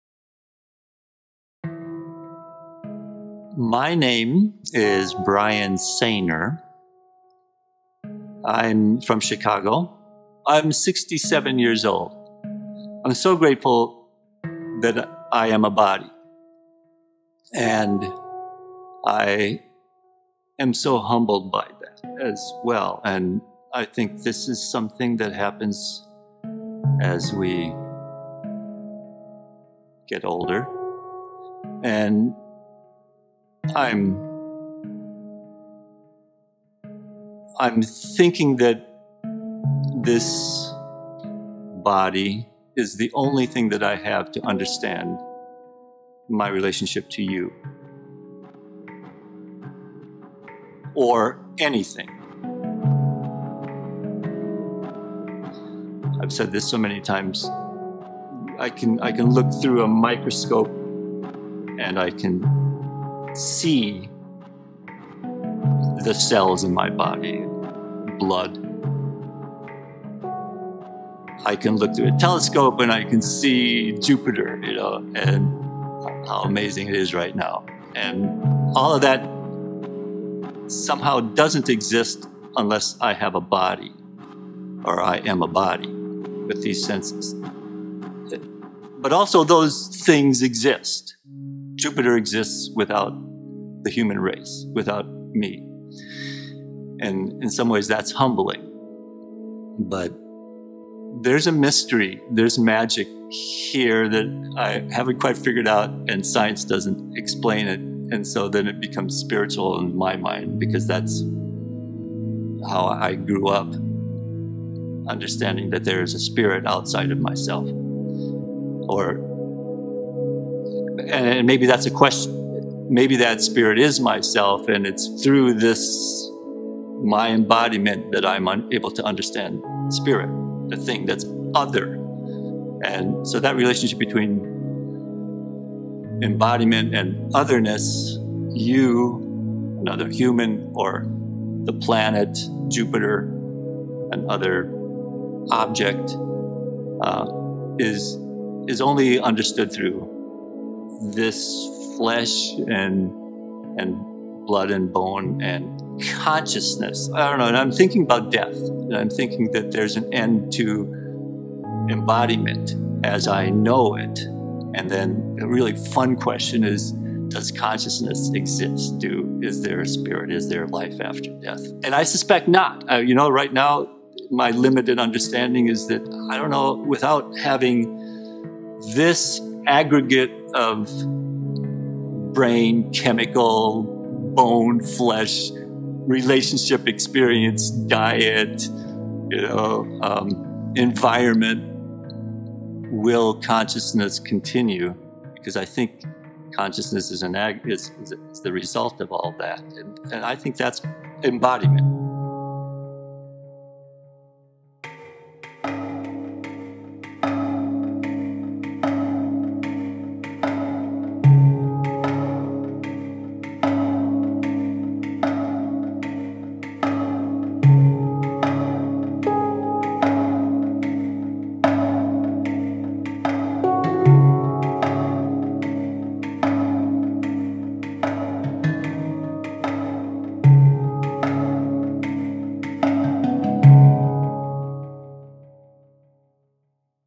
A Dance For A Time Being featuring Bryan Saner
A Dance For A Time Being interview with Bryan Saner

Dancers bodies accumulate insight and knowledge throughout years of training and life experience. This project asks us to reflect on the body as a Time Being, ever changing and growing, filled with embodied wisdom.
Some 30,000 years ago human beings first sketched out their own images in cave walls. What will the portraits of the future look like? How do we celebrate the everchanging human form and its expressive nature, and how can technology help us to experience and appreciate it in new ways?

Featured Artist: Bryan Saner

A Dance For A Time Being
Concept and Direction by Nejla Yatkin
Filmed by Enki Andrews
Edited by Enki Andrews and Nejla Yatkin
Augmented Reality and App Developer – Eugene Ovcharenko and team at intenvix

This project was made possible in part by 3Arts Project fundraiser, The Harris Theater and DCASE Esteemed Artist Award.